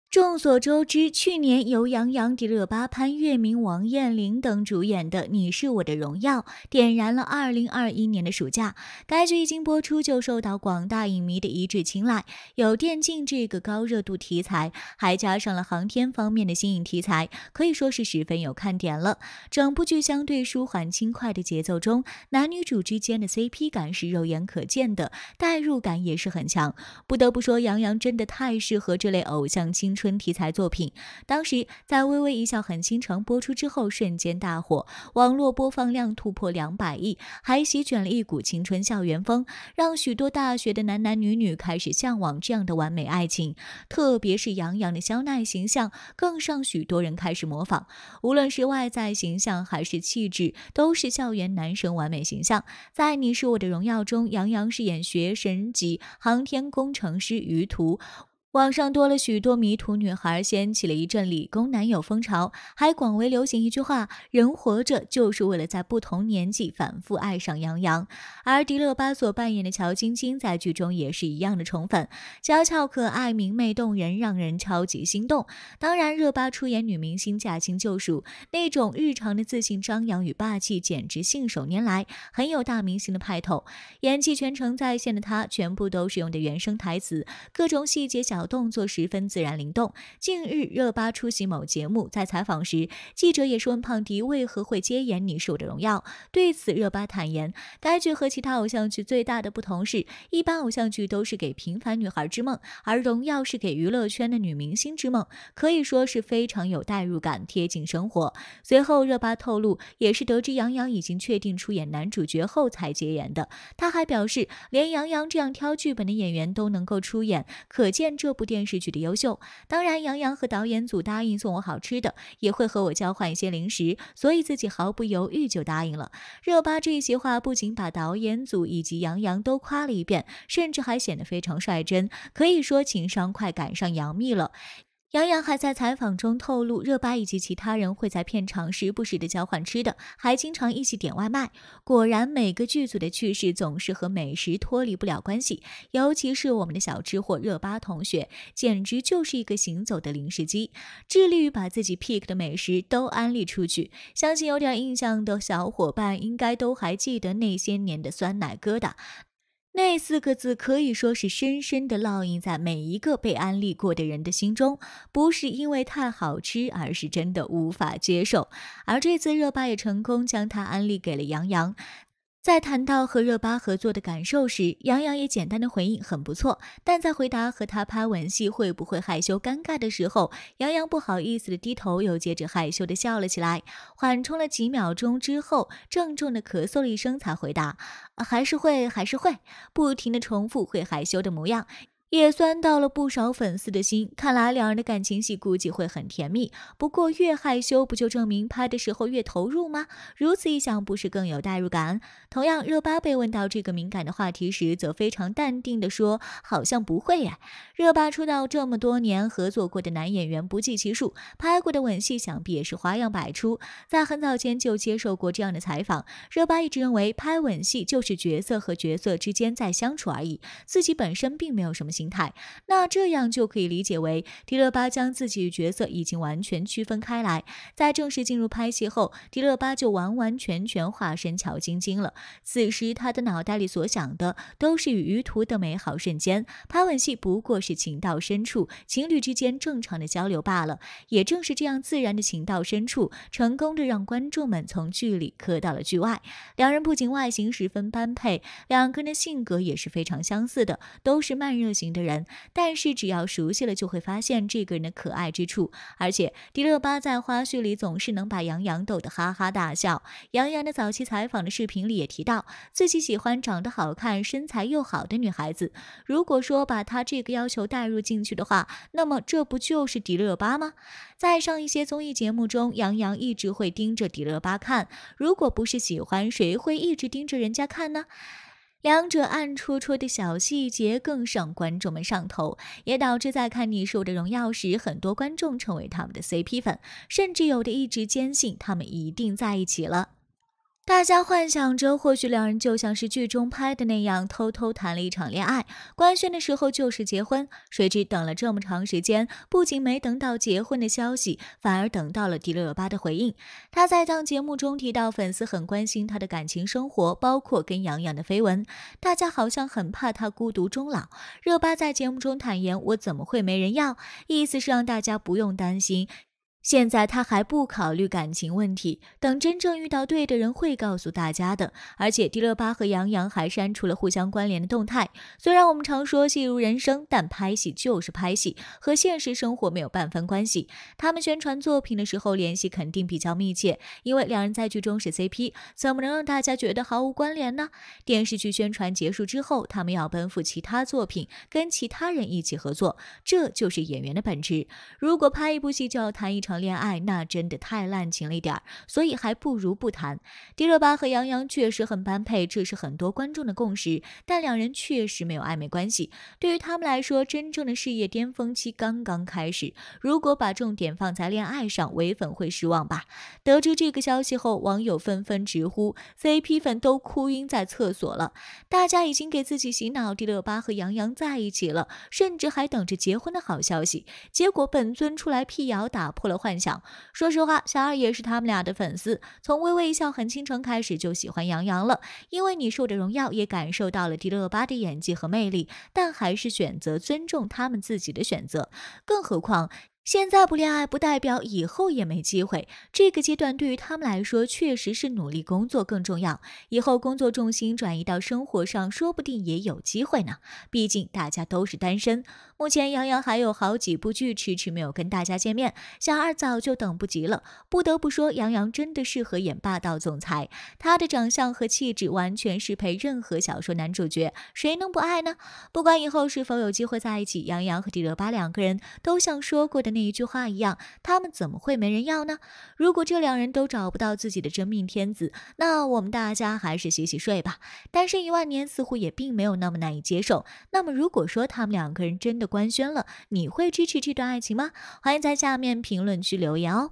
迪麗熱巴承認了？被問為何接演《你是我的榮耀》，脫口而出這兩字，暴露與楊洋真實關係！楊洋和熱巴這是雙向奔赴吧！ ？楊洋
眾所周知
去年
由楊洋、迪麗熱巴、潘粵明、王彥霖等
主演的《你是我的榮耀》點燃了2021年的暑假
該劇一經播出就受到廣大影迷的一致青睞
有電競這個高熱度素材
還加上了航天方面的新穎題材
可以說是十分有看點了
整部劇相對舒緩輕快的節奏中
男女主之間的CP感
是肉眼可見的
代入感也是很強！

迪麗熱巴承認了？被問為何接演《你是我的榮耀》，脫口而出這兩字，暴露與楊洋真實關係！楊洋和熱巴這是雙向奔赴吧！ ？